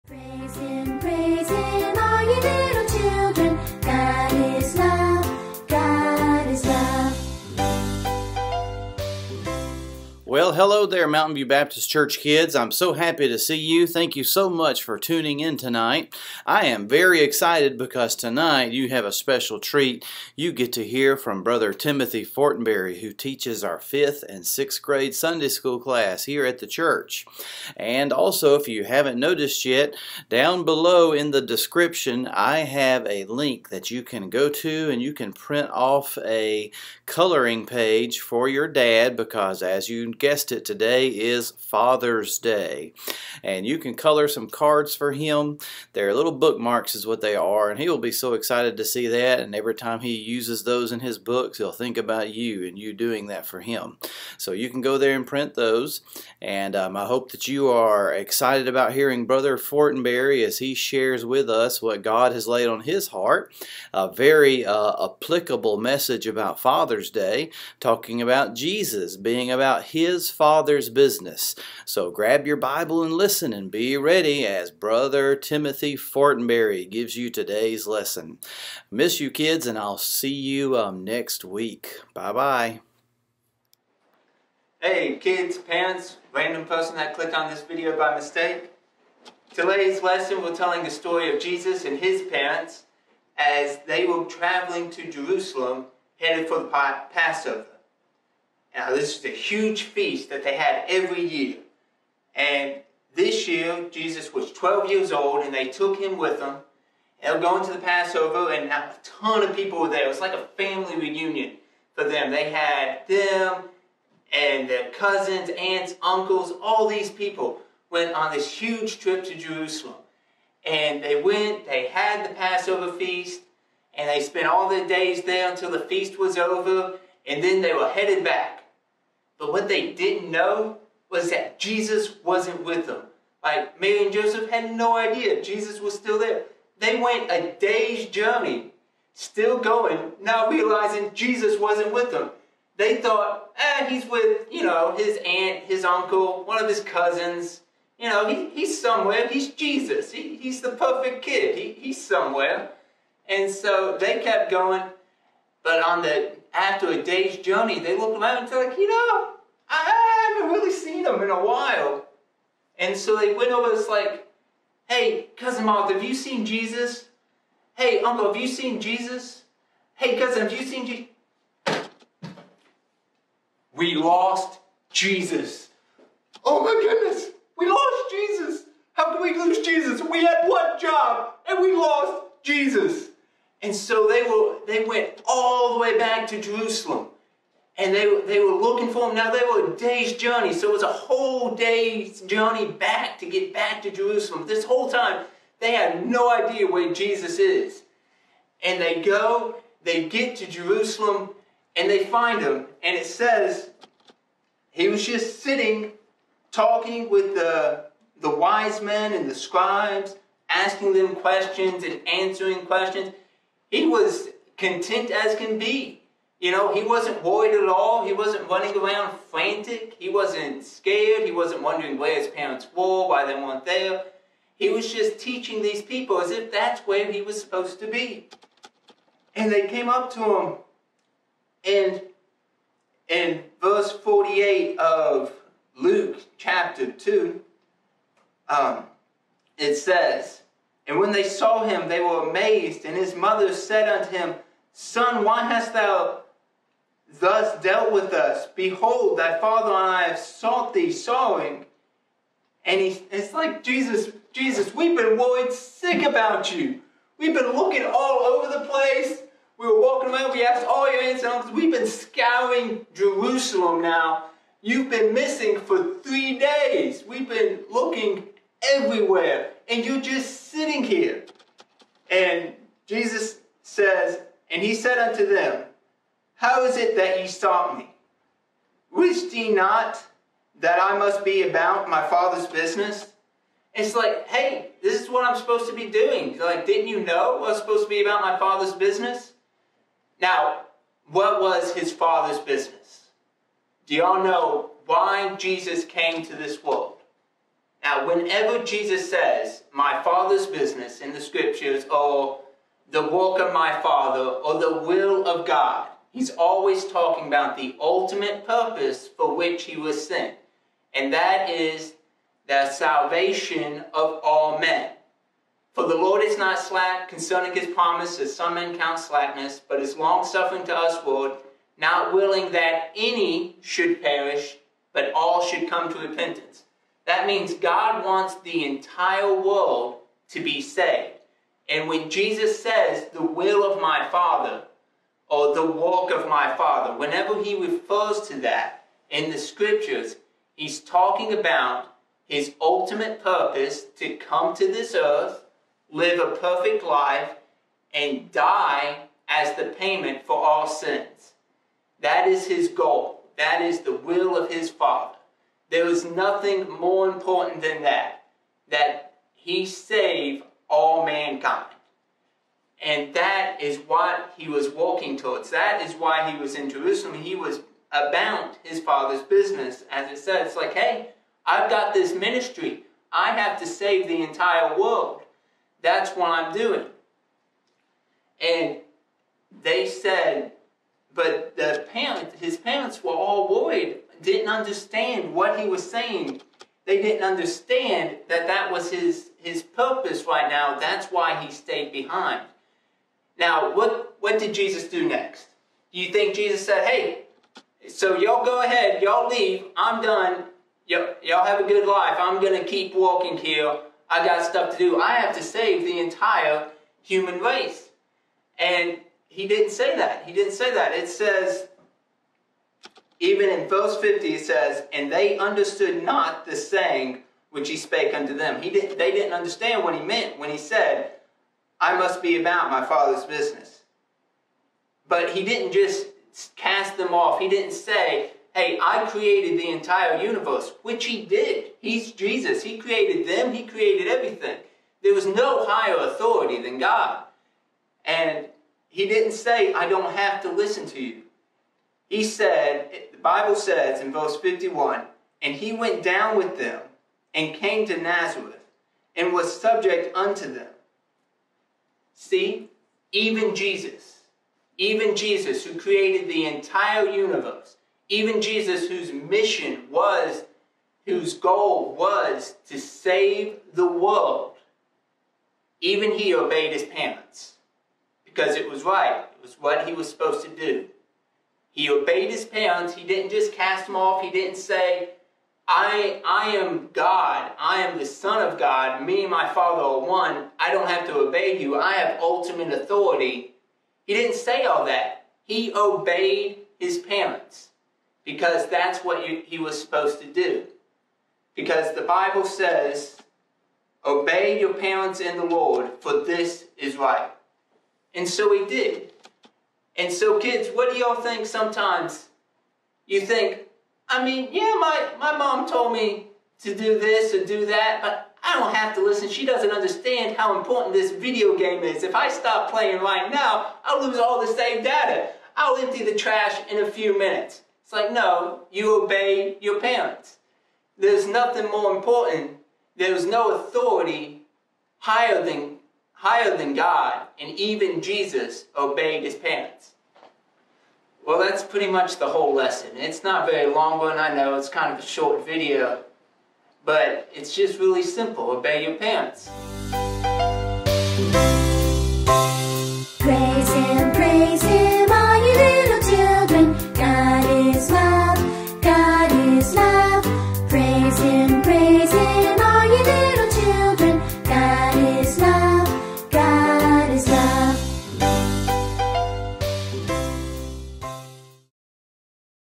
Sunday School, June 21, 2020
Hey Mountain View Baptist Kids! Please enjoy this Sunday School lesson by Brother Timothy Fortenberry!